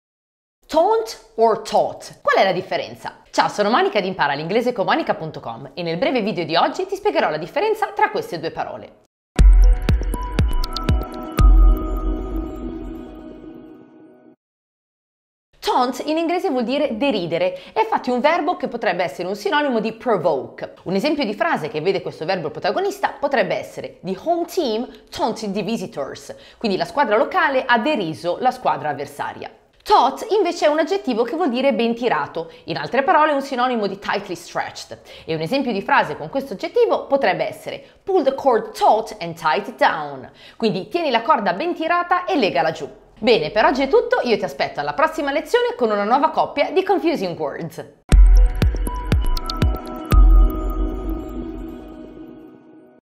TAUNT vs TAUT: Confusing Words
TAUNT or TAUT?
Qual è la differenza tra queste due parole?
Guarda ORA il mio video e memorizza la differenza!🤓

Riceverai per 4 giorni 1 lezione di estremo valore direttamente sulla tua email, per un totale di oltre 4 ore, a costo zero!